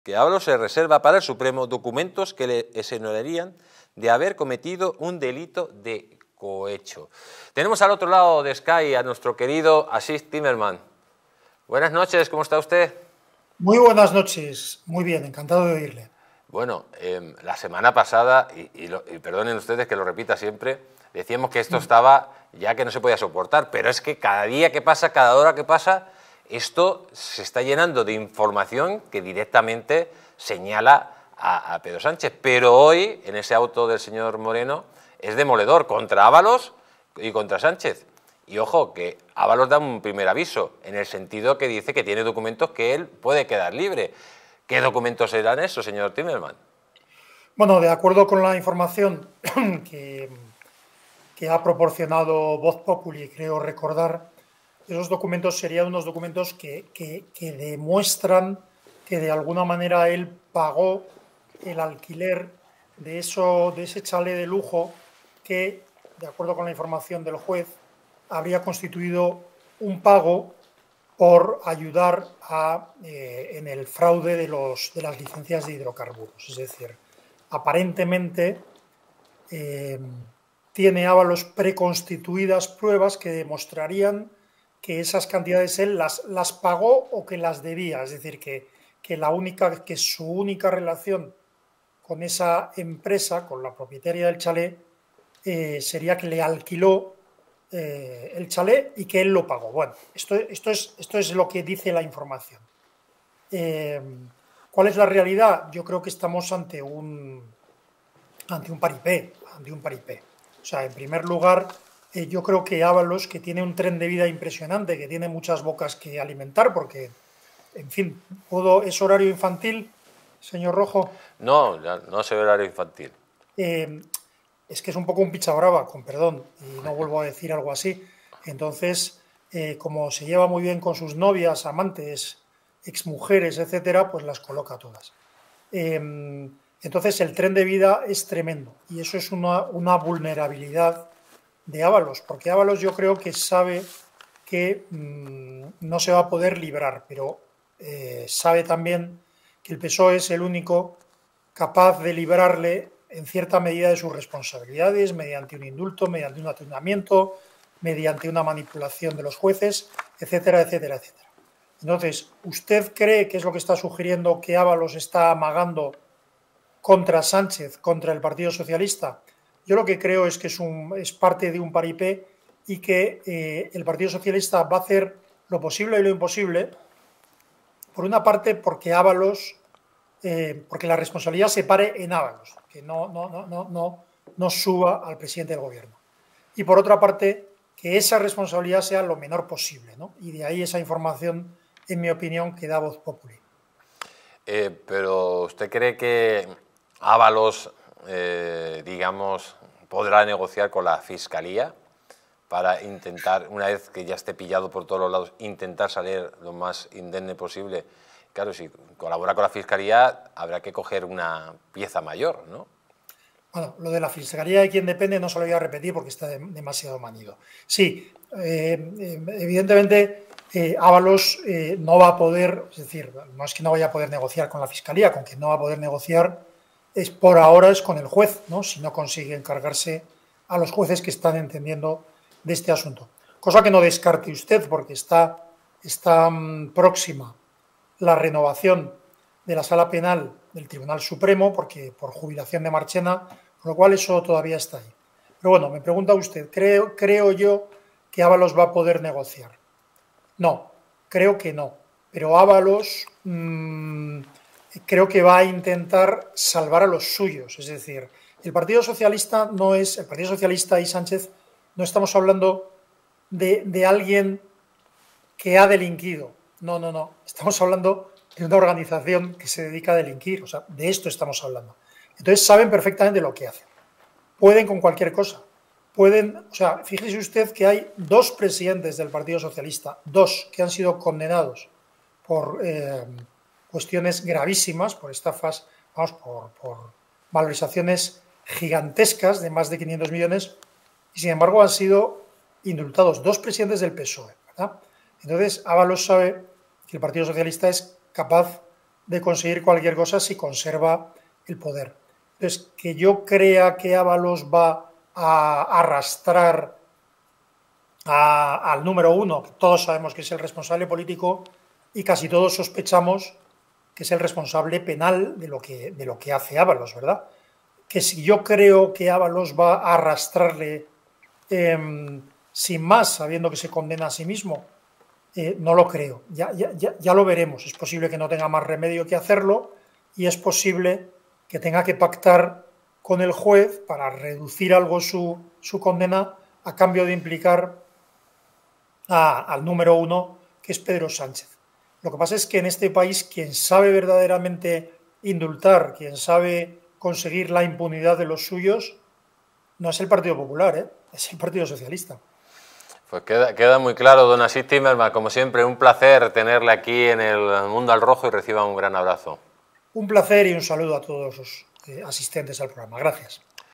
🔴BRUTAL🔴Ábalos reserva para el Supremo documentos que le exonerarían y que hunden a Sánchez
Ábalos guarda documentos clave para su defensa ante el Supremo, mientras que hunden a Sánchez. ¡No te pierdas este escándalo político en Distrito TV!distritotv
🔴Conviértete en miembro de este canal para disfrutar de ventajas: